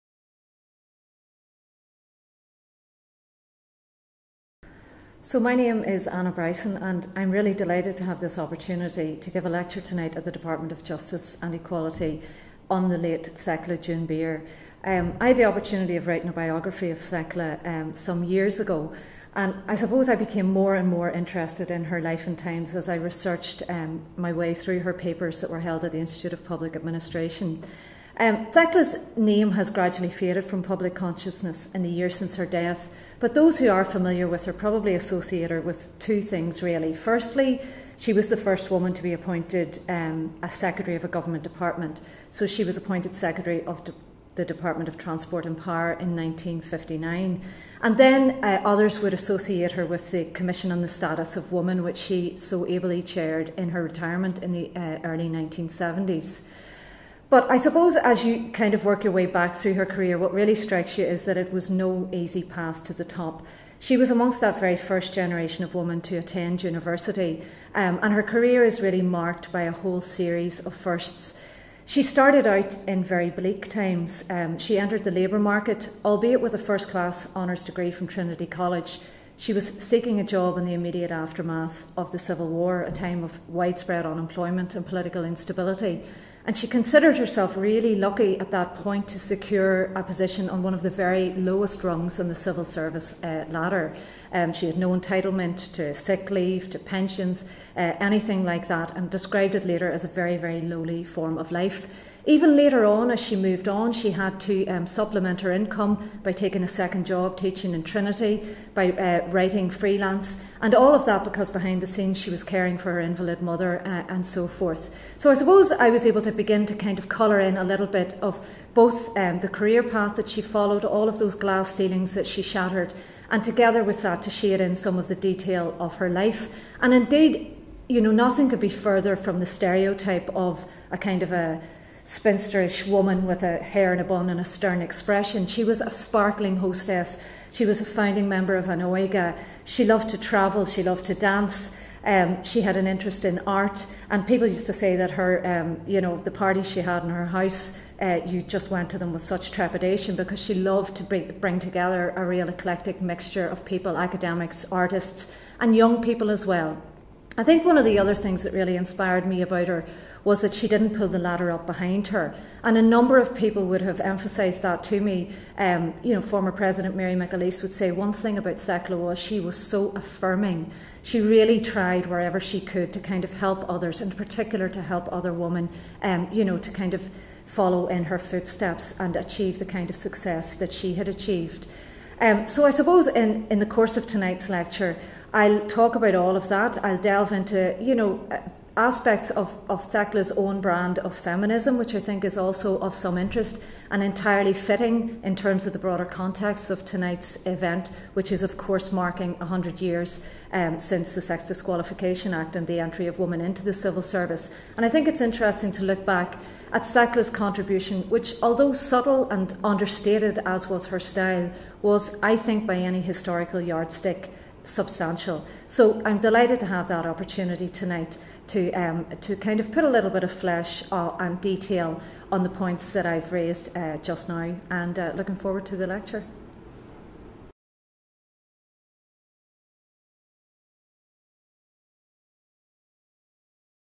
Thekla Beere - No Coward Soul
Guest lecture on Thekla Beere, the first female Secretary General of a Government Department, by Dr. Anna Bryson, author of No Coward Soul: A Biography of Thekla Beere. The lecture took place in the Department of Justice and Equality on Wednesday, 27th November 2019 as part of an event celebrating 100 years of women in the civil service and legal professions and marking the centenary of the Sex Disqualification (Removal) Act 1919.

Dr. Bryson provides an insight into Ms. Beere’s journey to becoming Secretary General of the Department of Transport and Power in 1959 and her substantial contribution to Irish life and society.